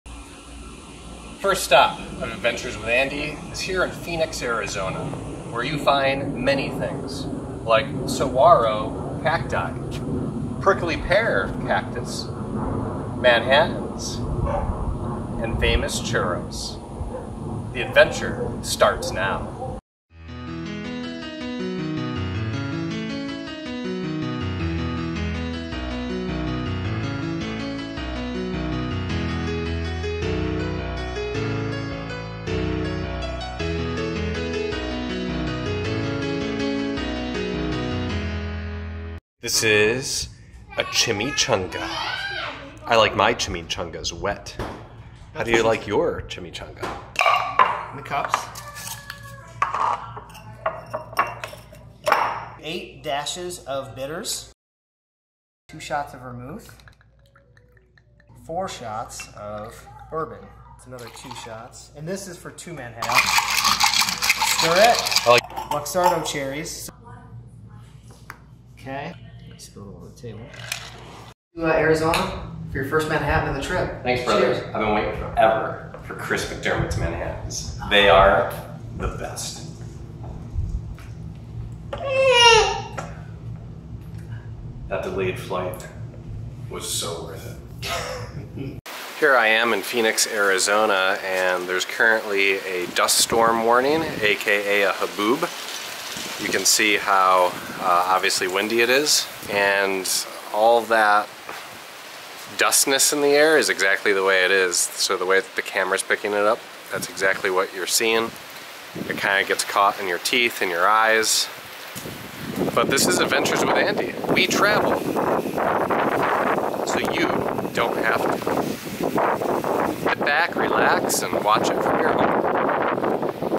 Phoenix Arizona
Wet Chimichanga's, Manhattan's, Saguaro Cacti, and being in a Haboob?!  The adventure starts NOW!